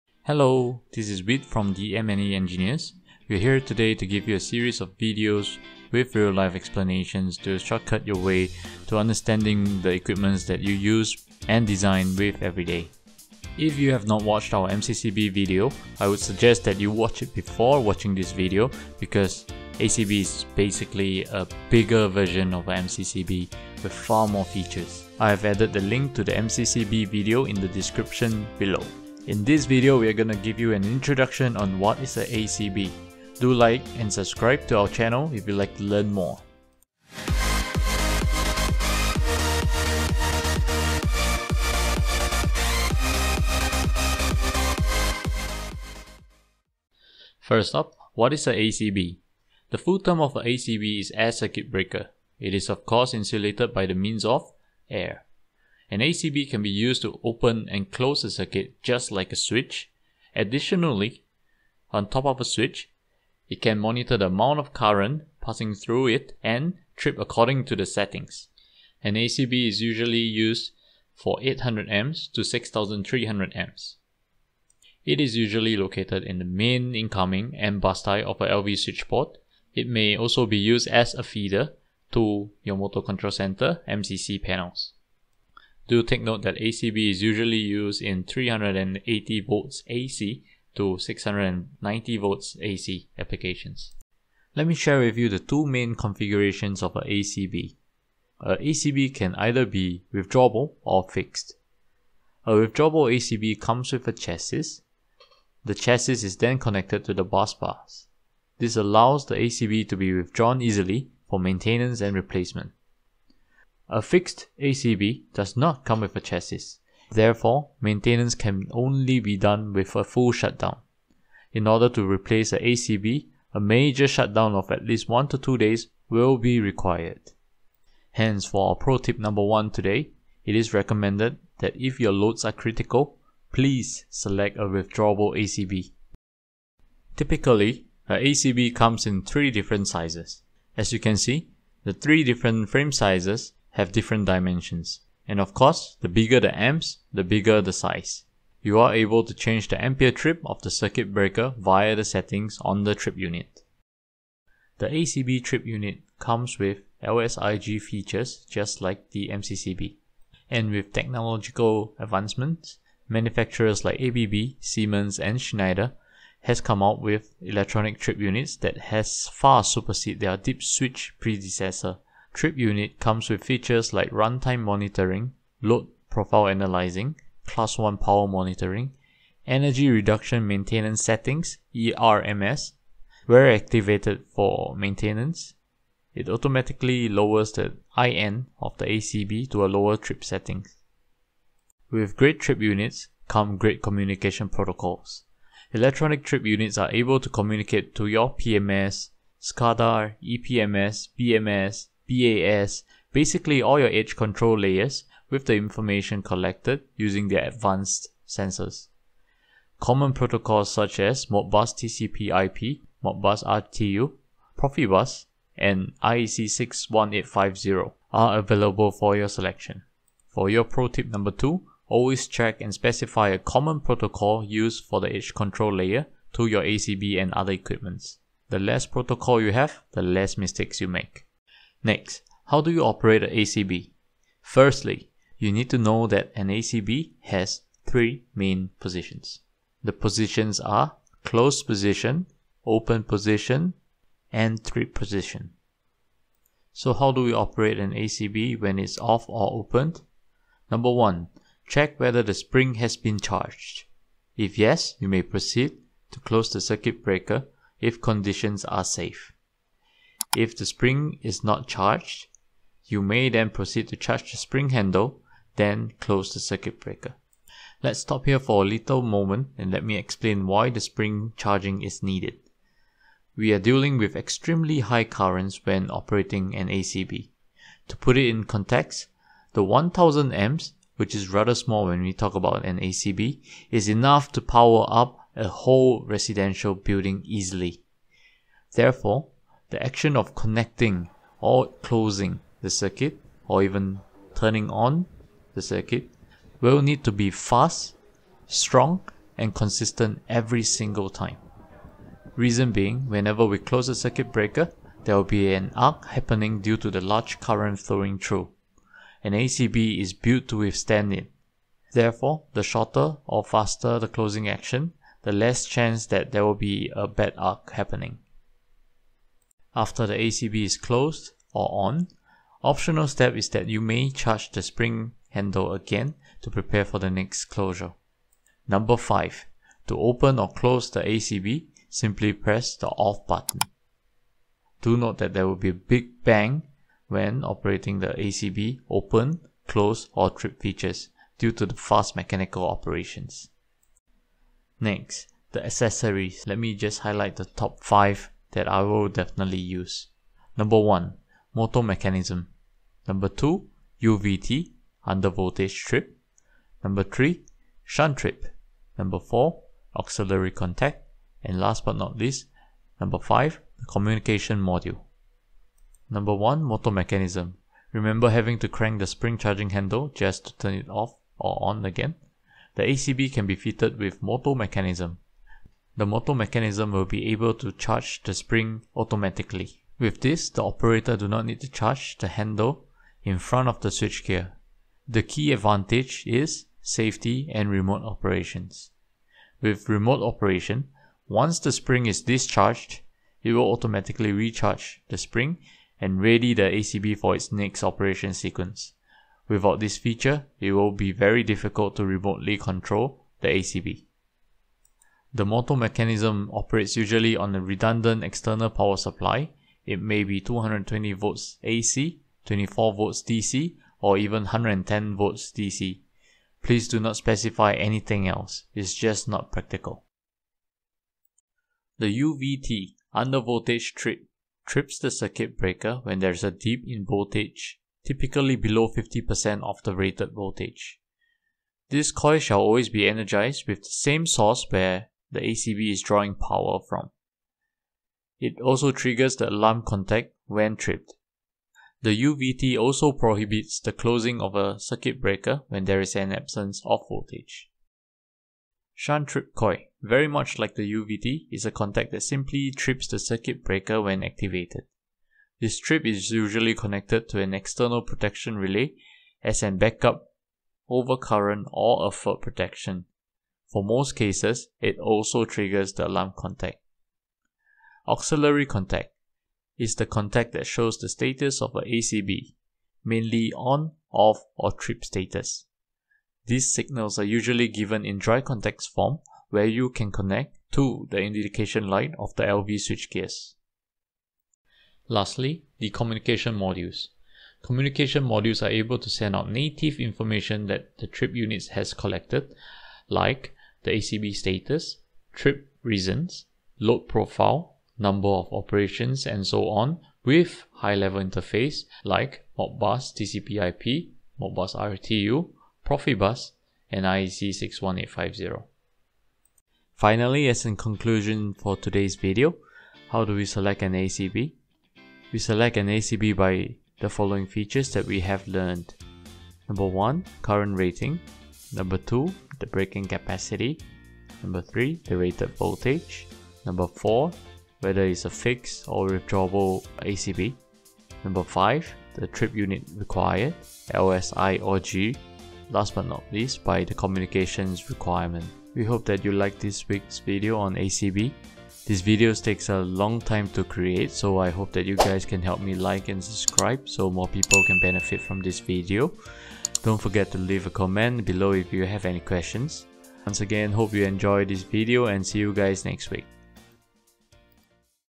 Episode 2 - What is a ACB, Air Circuit Breaker, Trip Units, draw-out explained by a M&E engineer!
This this video you will learn about:-
Introduction
Withdrawable / Fixed ACB?
Frame Sizes
Trip Units
How do you operate an ACB?
The Accessories
How do we select an ACB?
Conclusions

If you havent seen the first episode on Basics on MCCB and Circuit breakers we highly recommend you check it out before watching this!

More videos like this coming up!

Weet is a seasoned M&E engineer who has worked with major engineering MNCs. He is a electrical engineer by discipline.

Artwork: Credits to OEMs. Do contact to remove any licensed images.